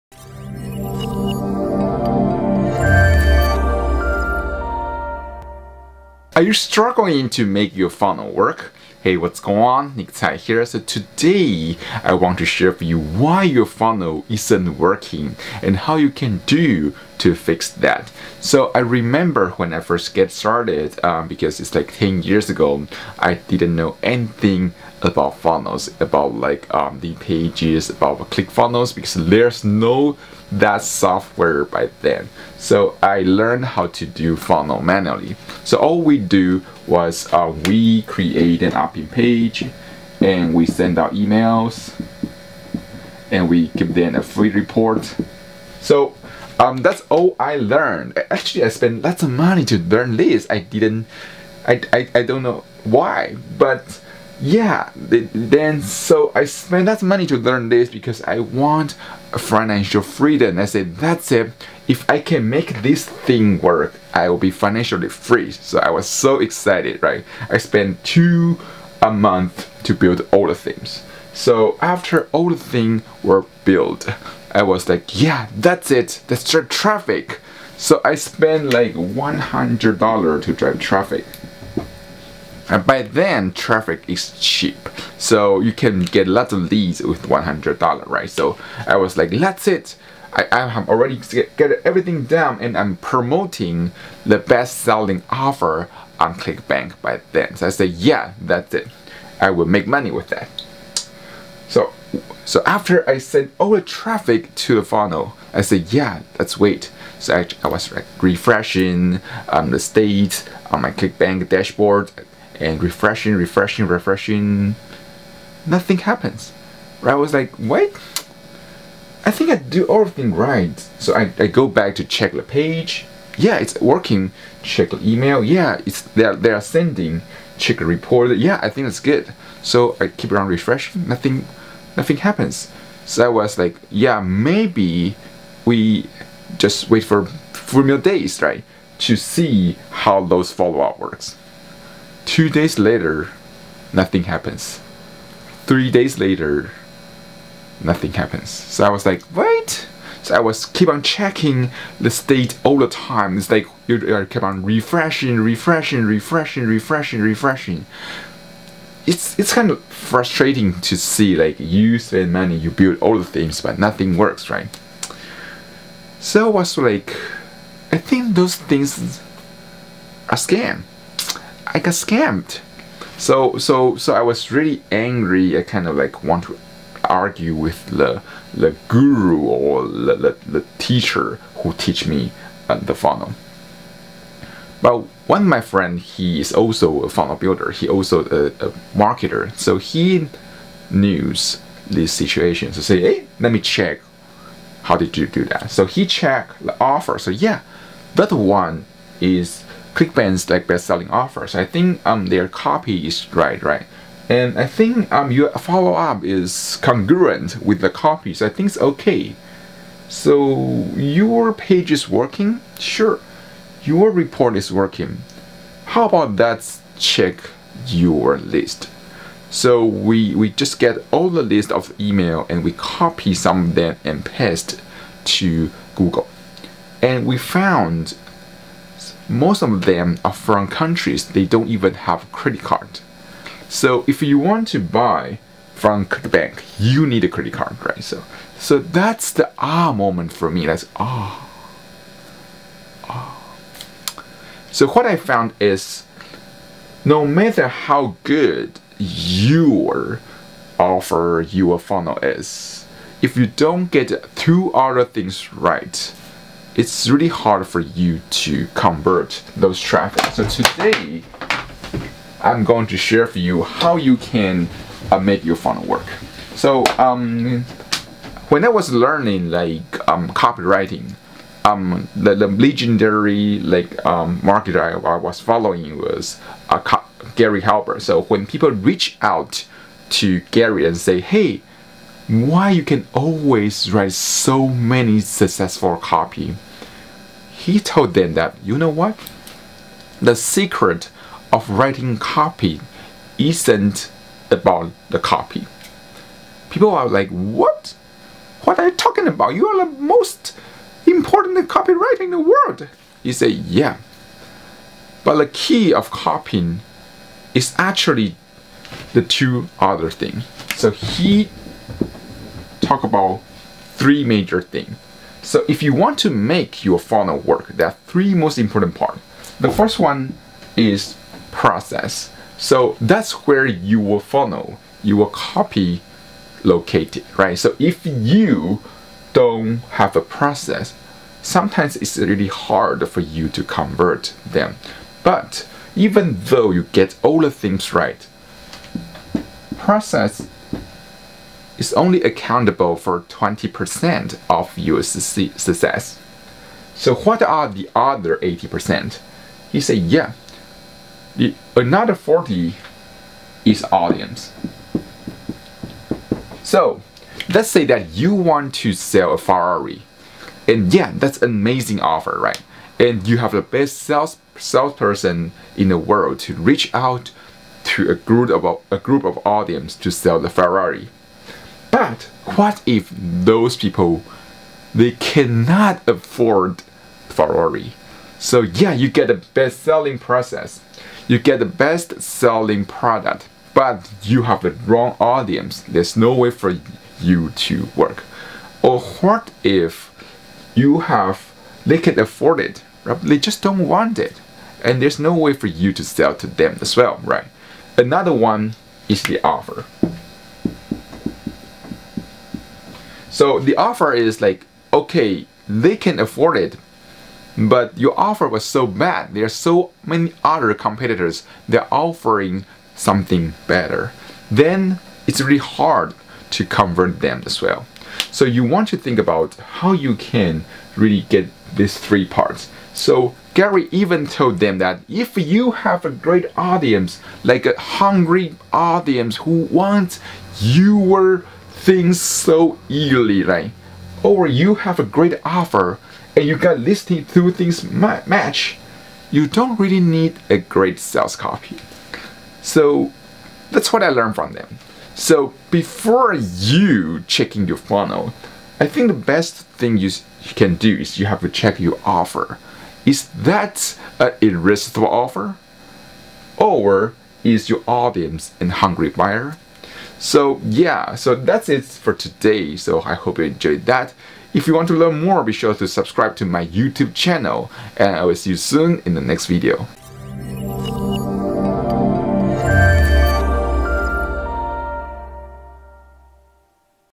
3 Reason Why Your Funnel Isn't Working? How To Build A Sales Funnel That Works Sales Funnel Strategy
Want to get more traffic and turn your following into your customers? Sign up for my free training here

-- Do you know what can you do to fix your funnel? Let me get you through this in this quick video.

-- I usually used to create opt-in page. Send emails. Through this, I wanted to earn money and wanted to be financially free. I spent $100 for traffic back then it was really cheap. Was working on Clickbank. My funnel stopped working and I was trying to Refresh it but it wasn't working also. I waited for many days and kept refreshing the funnel so that it may work. I kind of got frustated with this. I was having a feeling that I think I have got scammed. One of my friend who is also a funnel builder. Came and checked my funnel and he said how about to check your email list. When we googled it,  we came to know that most of them who signed up are from countries who don't even have credit card.

-- There are three major parts in this game.
-Process - 20%
- Audience - 40%
- Offer - 40%

-- If you're offering Ferrari then yeah that's a great offer but many people just can't afford this. And sometime they can afford it but there offer is so bad. that they will not get signed up for you. Working on offer is the key element.